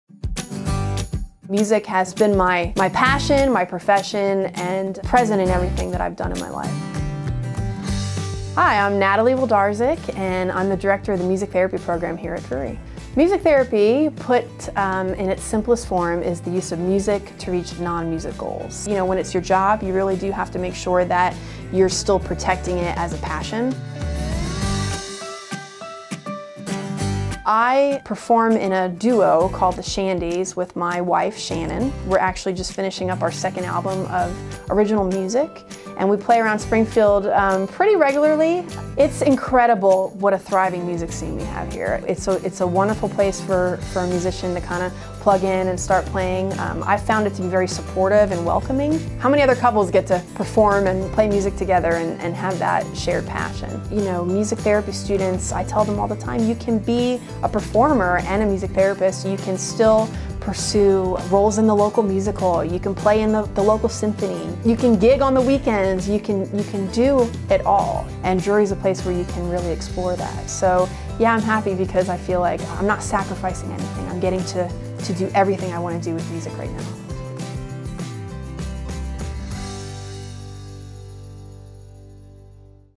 Passion + Professors: Dr. Natalie Wlodarczyk
Drury University professors live out our values of blending passion and profession. Dr. Natalie Wlodarczyk is a professor of music therapy by day and a recording artist and performer in the duo The Shandies on her own time. She says playing professionally helps fuel her passion, which she brings back to the classroom when mentoring her students.